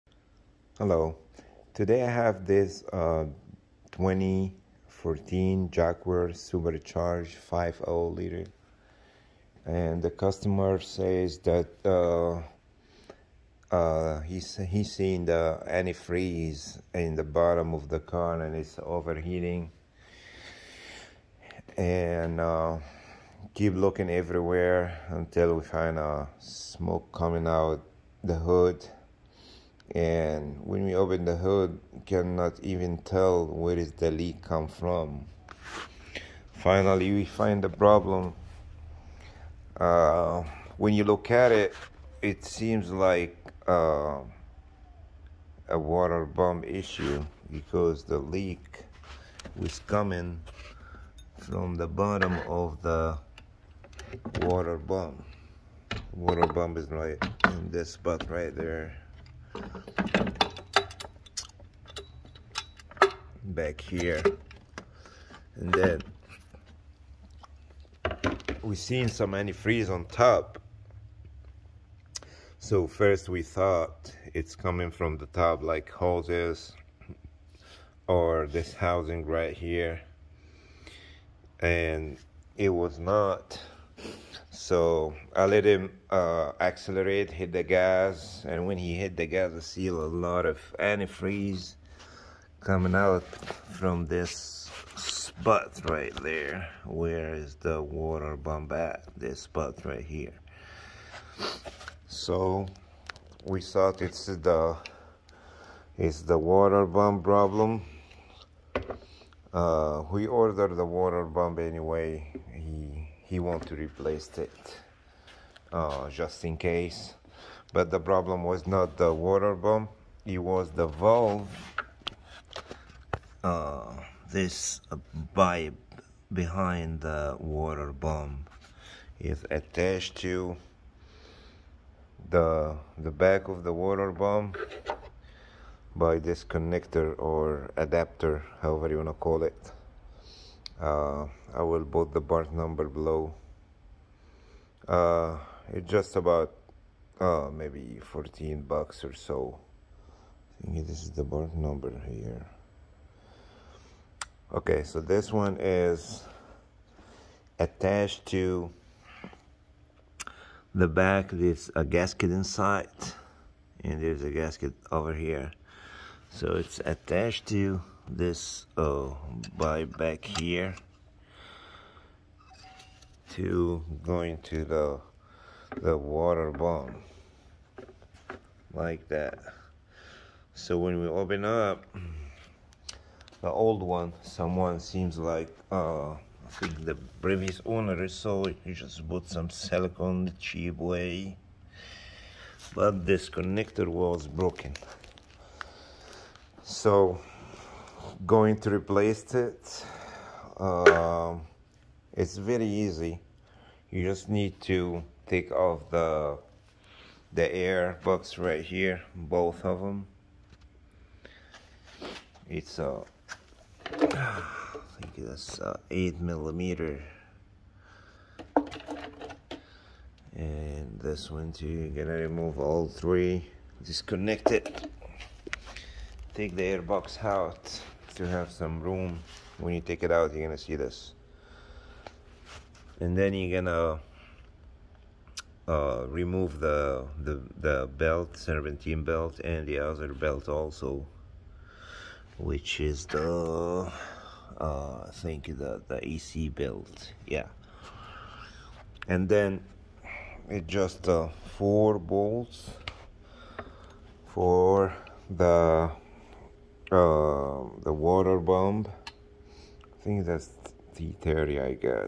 Jaguar & Land Rover 5.0l overheating and coolant leak (FIXED)
2014 Jaguar XJL Supercharged  5.0L

[1]  water pump Connector only LR028136 for Jaguar for Land Rover

[2] water pump and connector